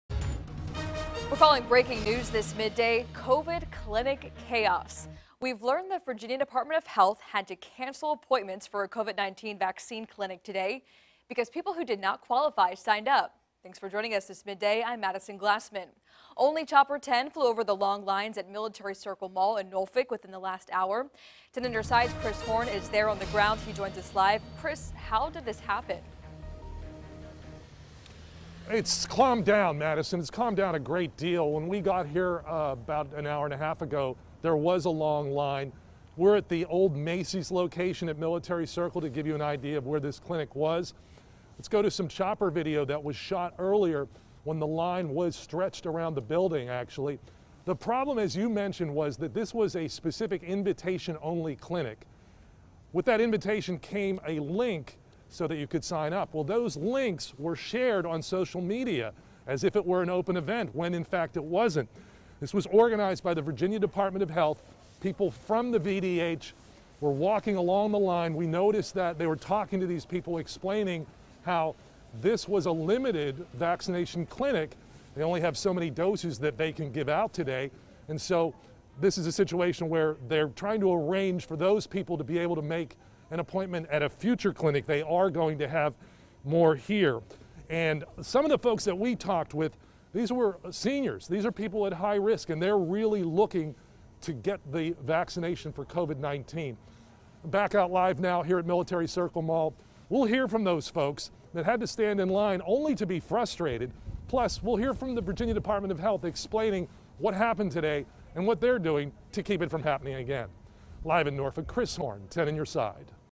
Long lines at Norfolk COVID-19 vaccine clinic; some turned away
10 ON YOUR SIDE LEARNED THE VIRGINIA DEPARTMENT OF HEALTH HAD TO CANCEL APPOINTMENTS FOR A COVID-19 VACCINE CLINIC TODAY BECAUSE PEOPLE WHO DID NOT QUALIFY SIGNED UP. CHOPPER 10 FLEW OVER THE LONG LINES AT MILITARY CIRCLE MALL IN NORFOLK. REPORTER CHRIS HORNE WAS ALSO THERE AND FILED THIS REPORT.